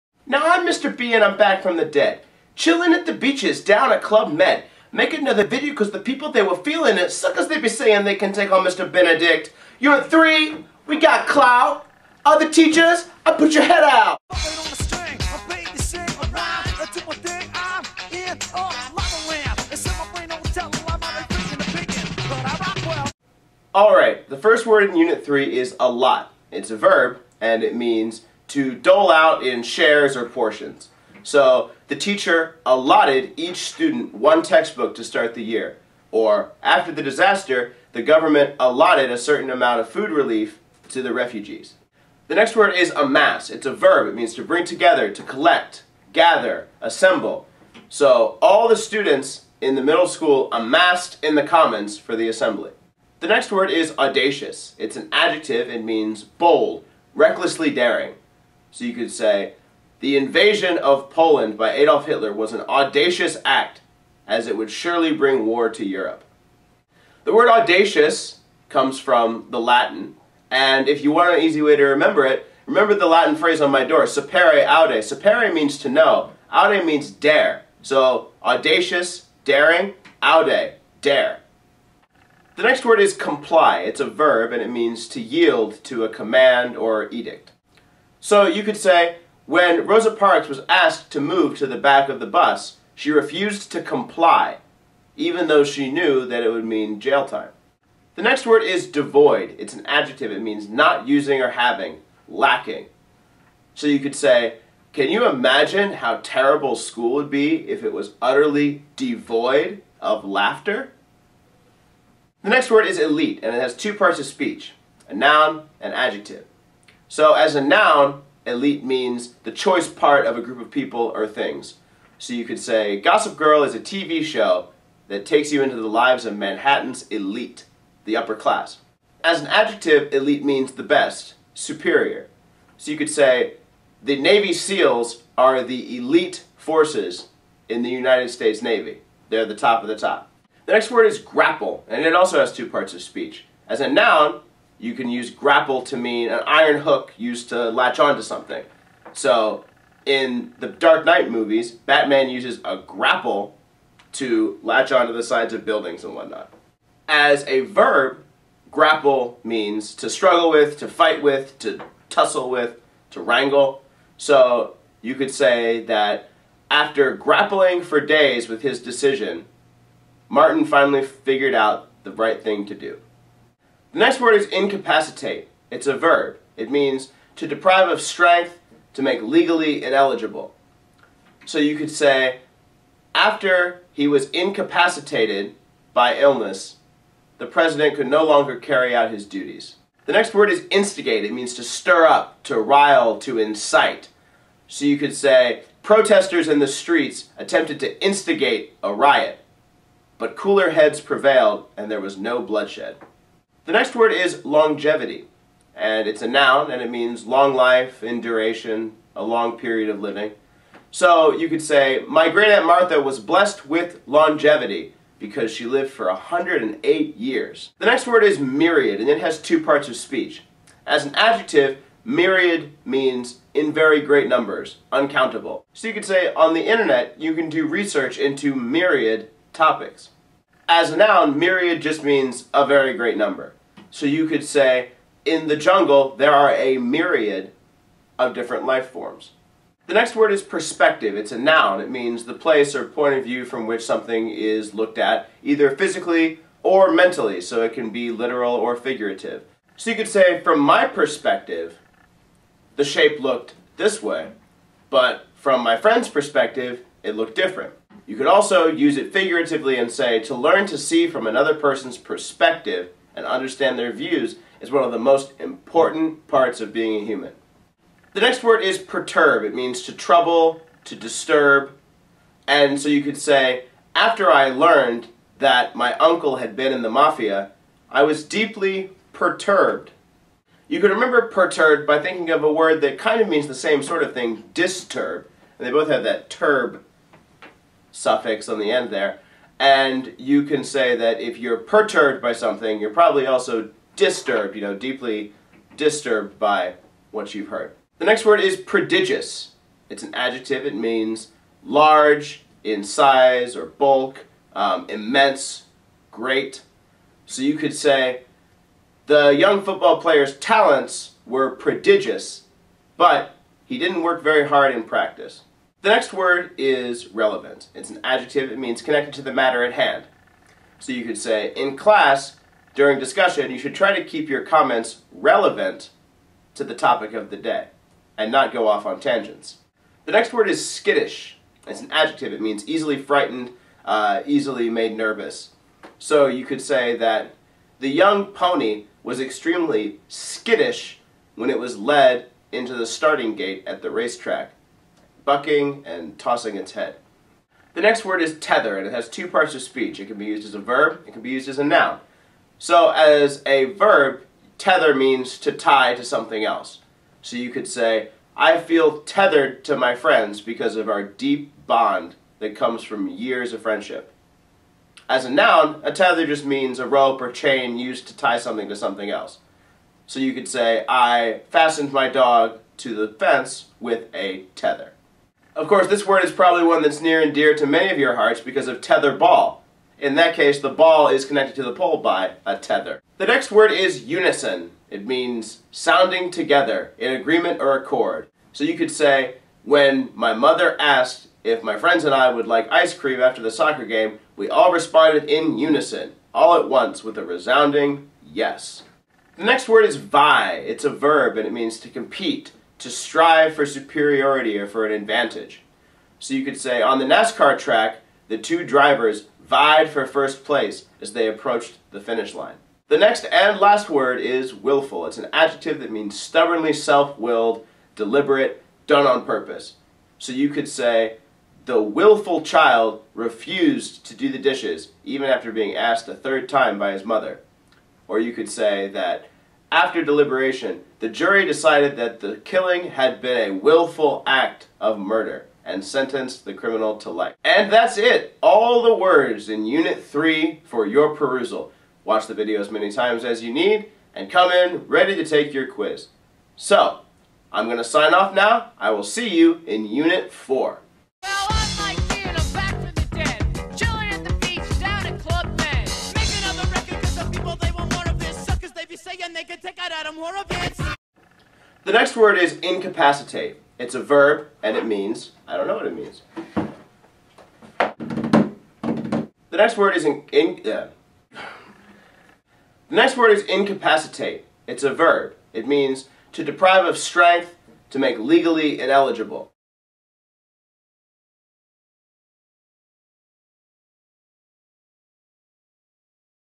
Mr. Benedict's Vocab Video (English 7, Unit 3)
Mr. Benedict takes you through Unit 3 of the vocab book.